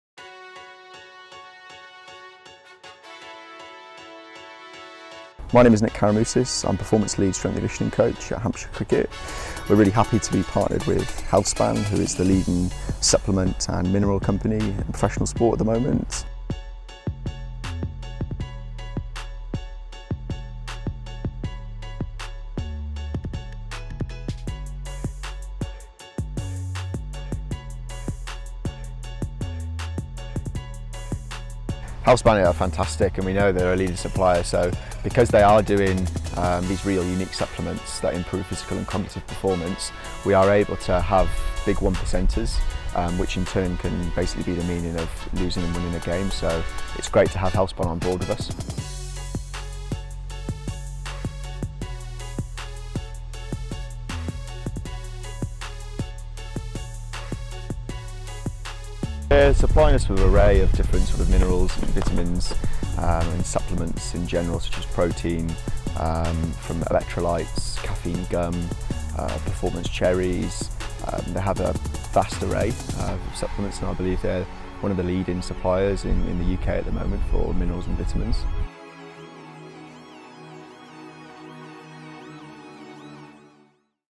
Healthspan: Official Nutrition Partner Of Hampshire Cricket
Hampshire are proud to be working with Healthspan, the leading supplier in sports nutrition and supplements.